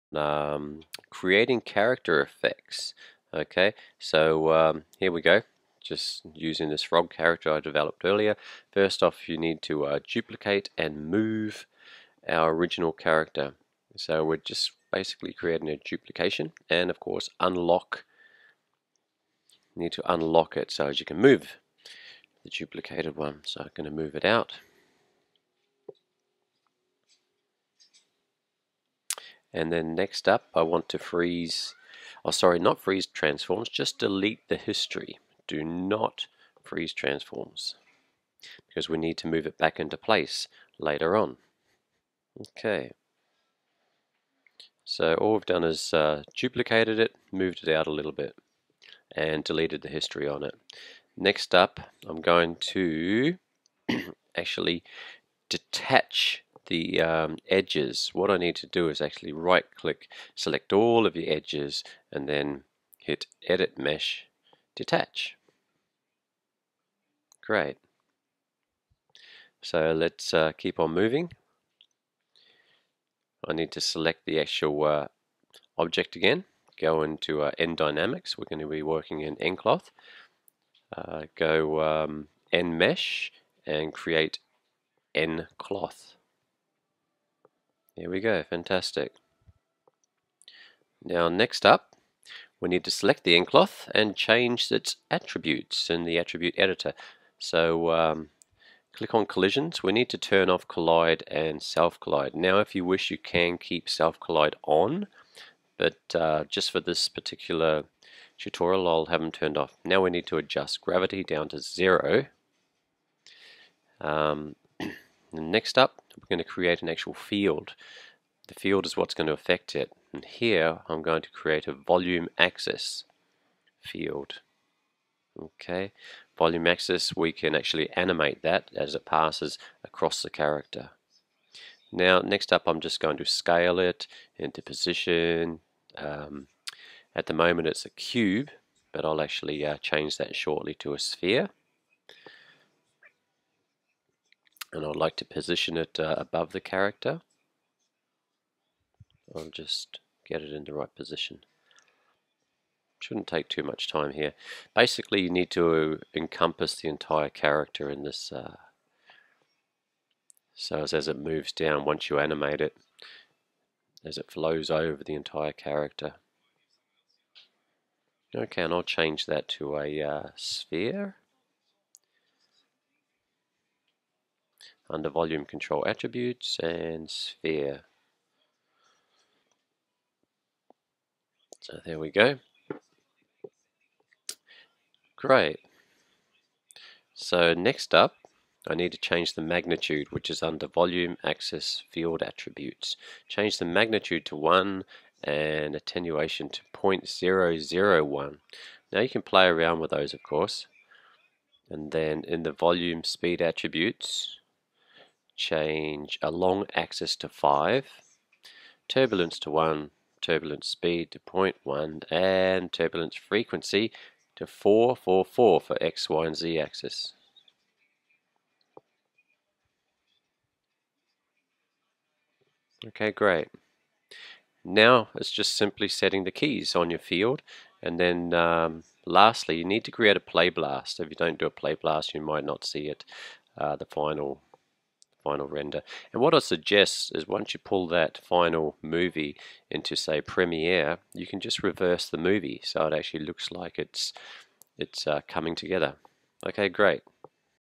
Maya Character Effect nCloth
Maya tutorial: how to make a  character's mesh explode into pieces.